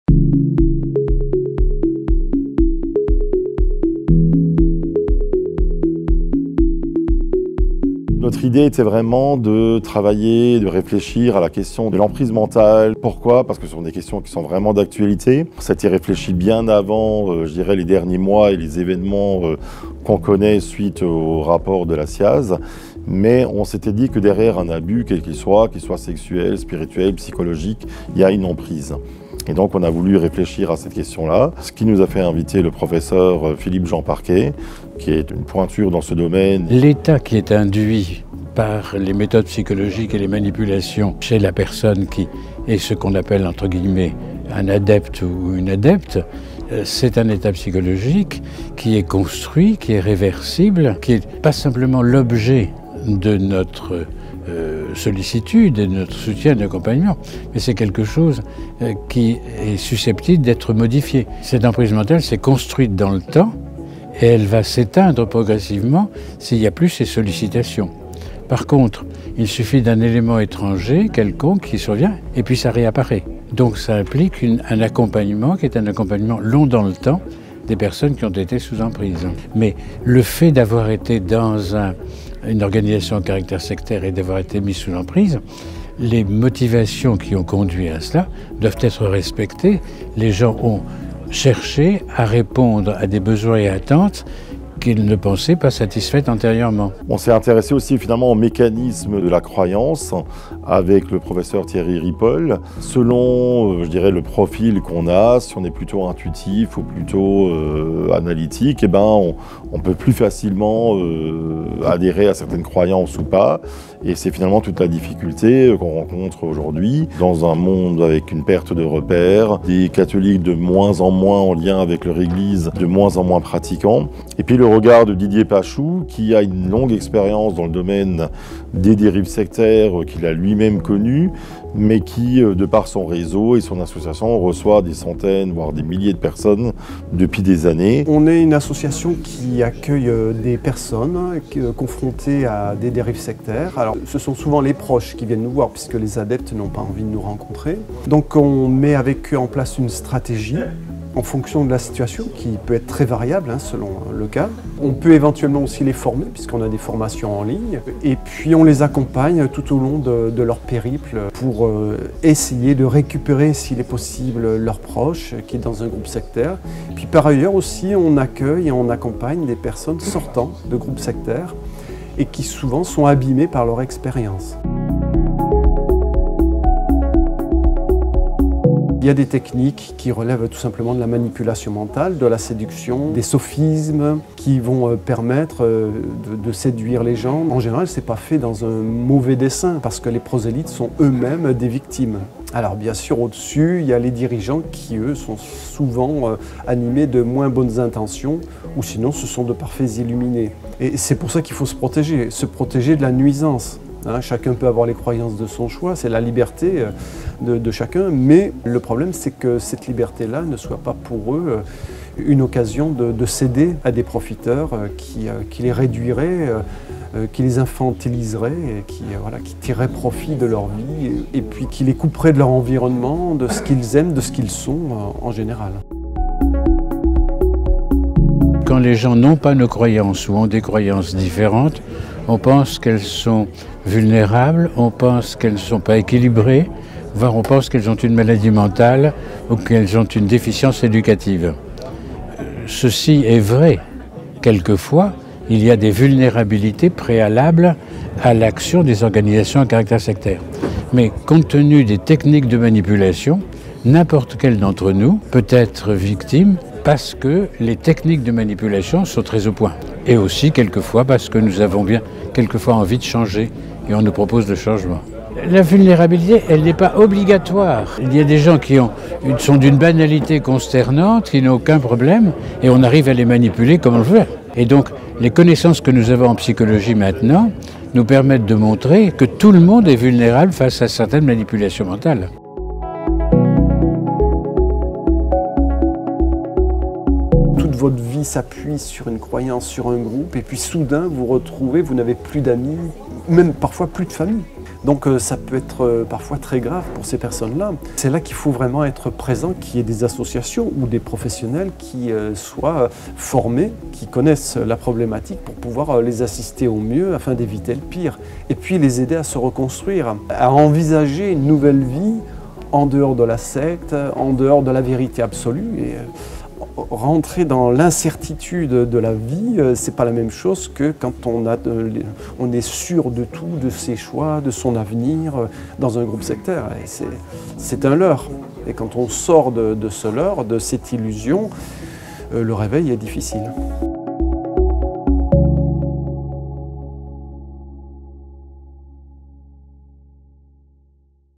Emprise mentale, croyances sectaires et radicales : la parole aux experts
Interviews de M. Francis Staehler, du Pr Philippe-Jean Parquet et de M. Didier Pachoud, dans le cadre d'une session de travail sur  l'emprise mentale, les croyances sectaires et radicales. Des clés pour comprendre la genèse des abus et des croyance infondées.

La session a eu lieu a la Conférence des évêques de France le 16 et 17 décembre 2021.